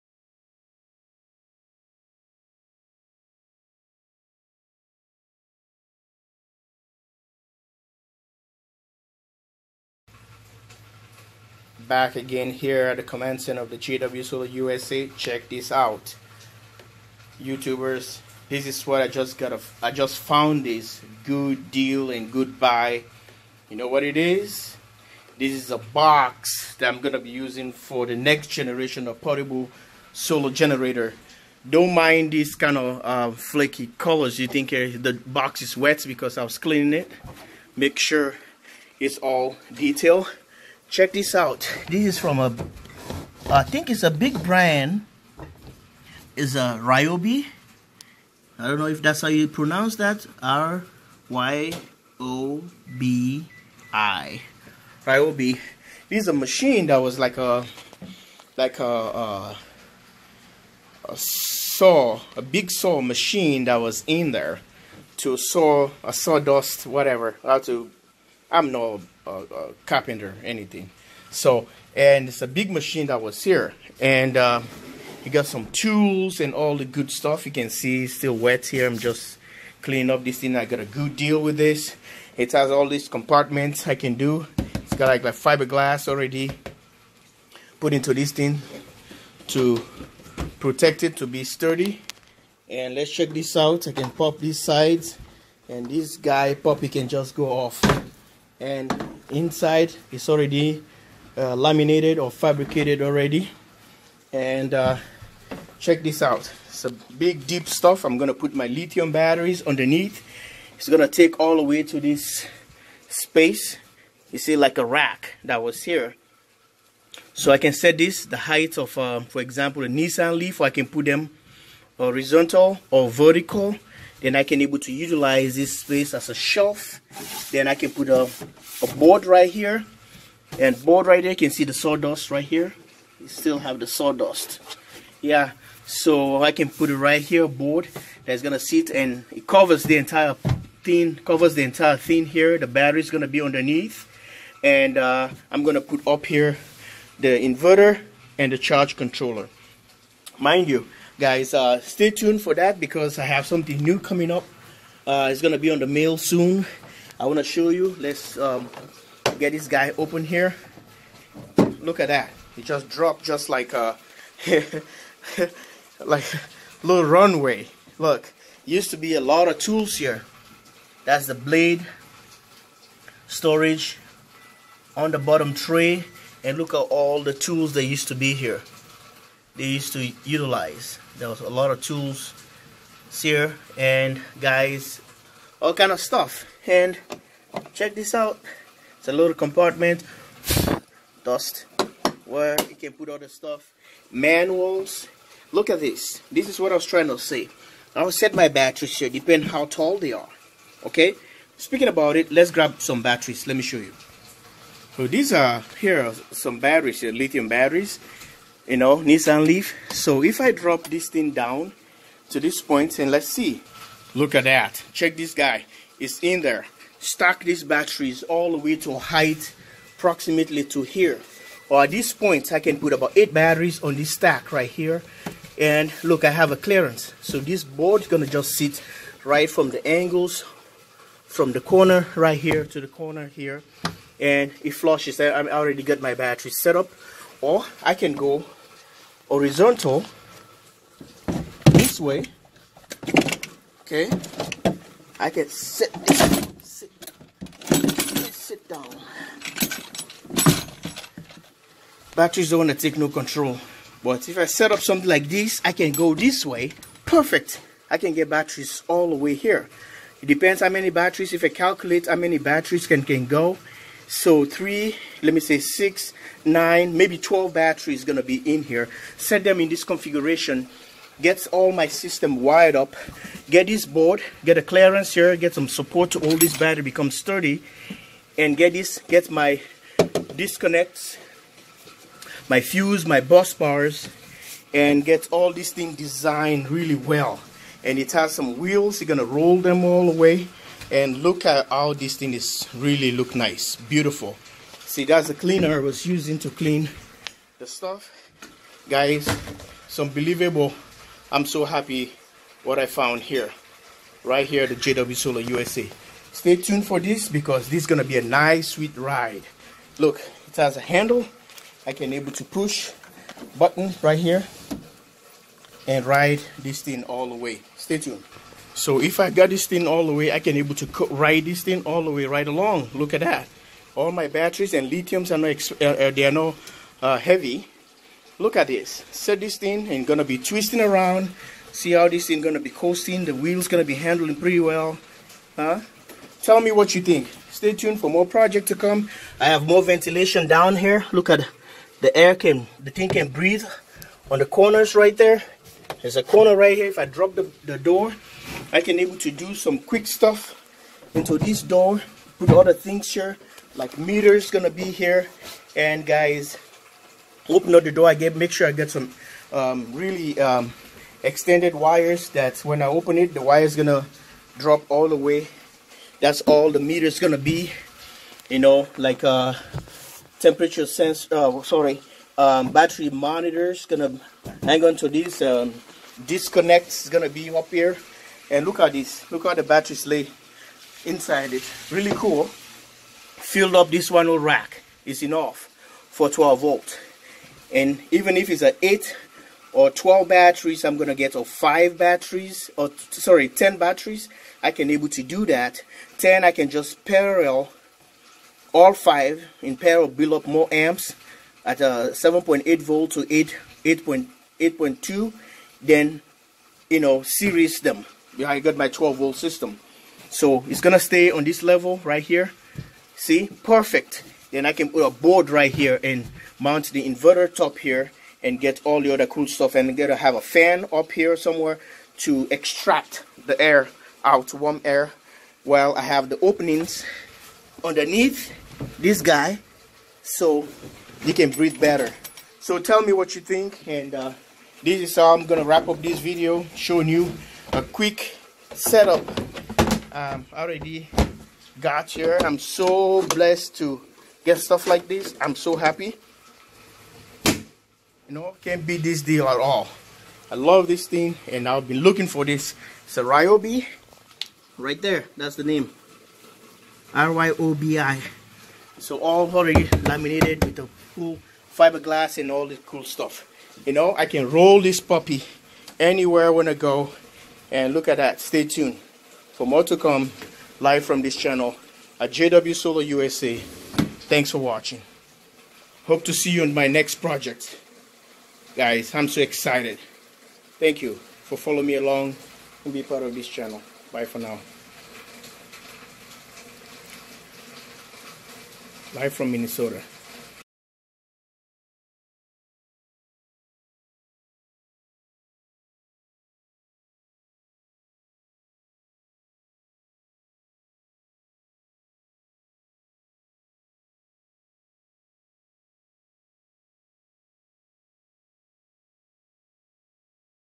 RYOBI TECHNOLOGIES INC: Fabricating Ryobi Tool Box for Portable Generator
How To Build A Portable Generator With A RYOBI Tool Box

Here's a link to my other:
DIY PORTABLE SOLAR POWER GENERATOR